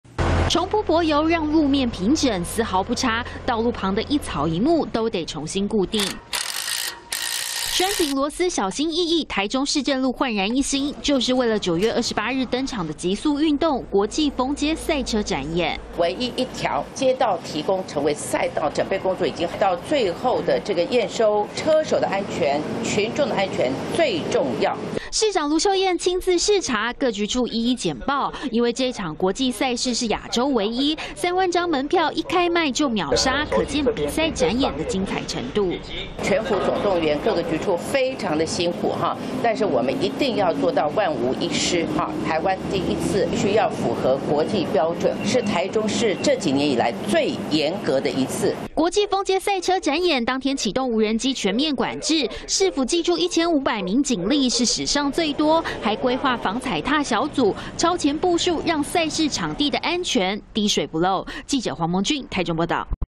國際封街賽車展演9/28台中登場 盧秀燕視察場地│中視新聞 20240907
國際封街賽車展演，9月28日在台中的市政路登場，市府超前部署整備場地，市長盧秀燕親自視察驗收。